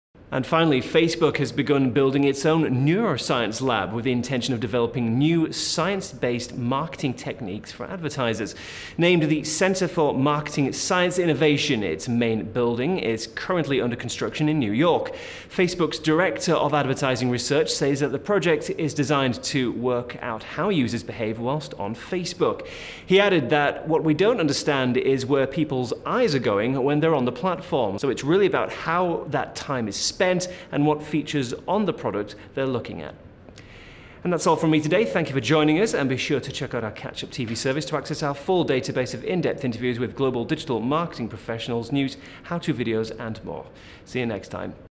Facebook starts building neuroscience lab
Facebook has begun building its own neuroscience lab, with the intention of developing new, science-based marketing techniques for advertisers.

Named ‘The Centre for Marketing Science Innovation’, its main building is currently under construction in New York.

Facebook’s director of advertising research says that the project is designed to work out how users behave whilst on Facebook.

He added that “what we don’t understand is where people’s eyes are going when they’re on the platform… so this is really about how that time is spent and what features on the product they’re looking at”.